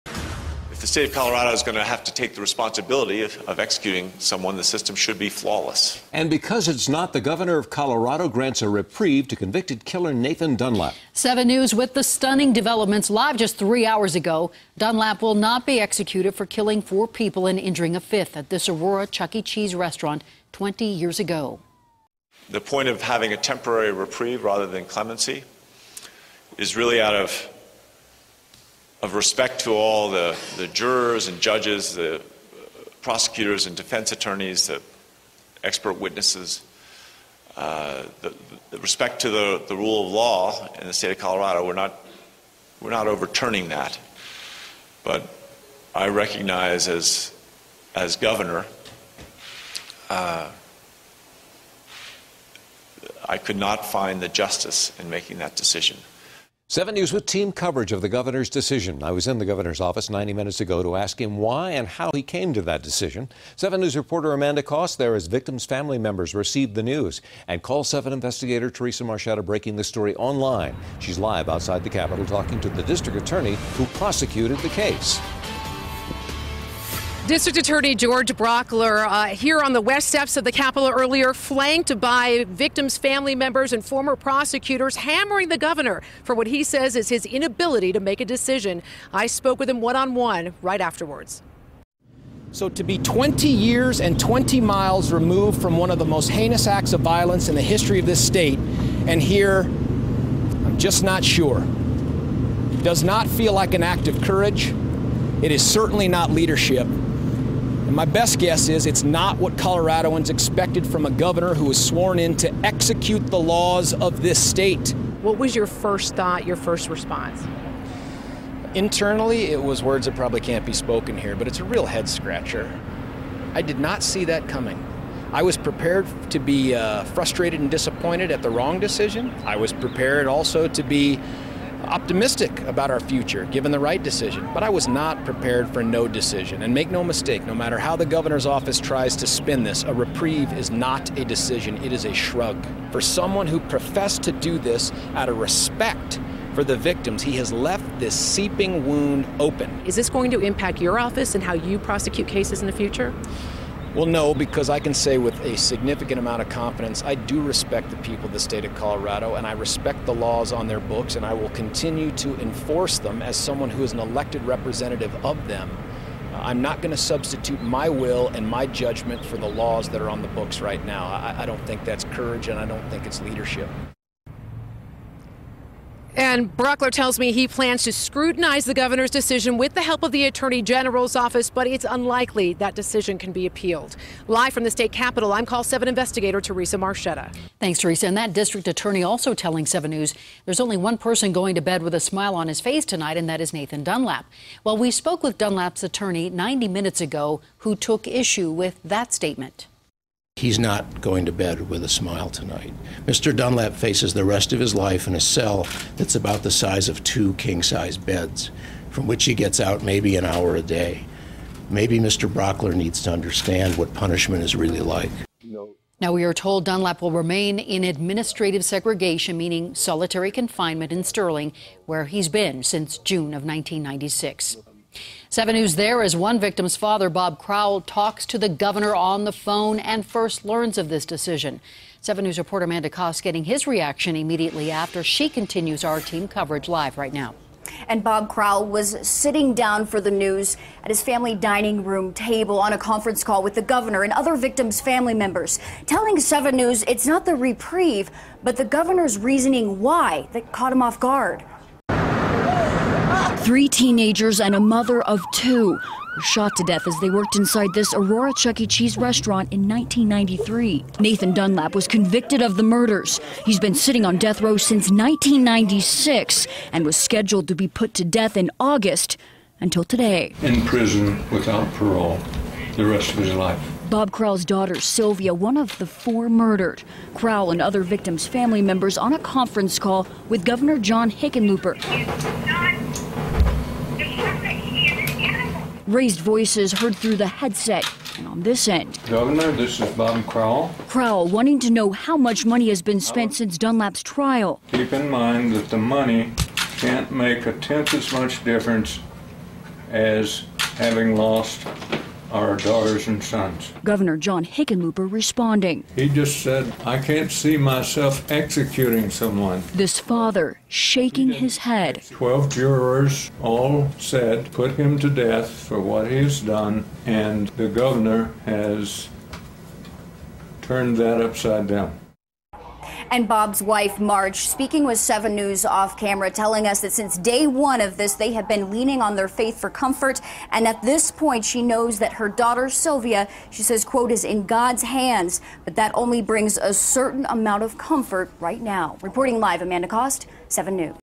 Convicted killer Nathan Dunlap gets death sentence reprieve
5/22 5 pm -- The sentenced to death for killing four people at a Chuck E. Cheese gets a reprieve from Gov. John Hickenlooper.  Watch our 7NEWS team coverage.